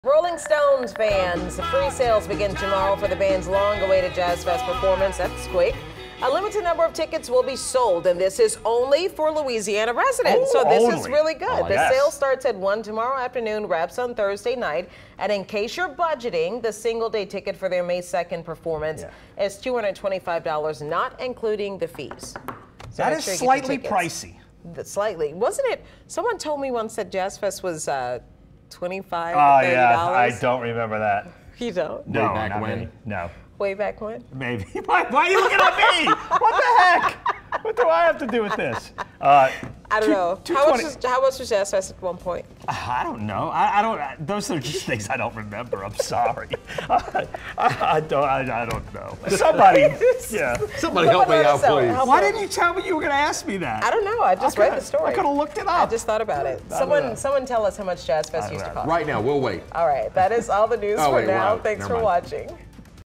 Presale tickets for Rolling Stones Jazz Fest performance
The sale starts at 1 p.m. Wednesday and wraps on Thursday night.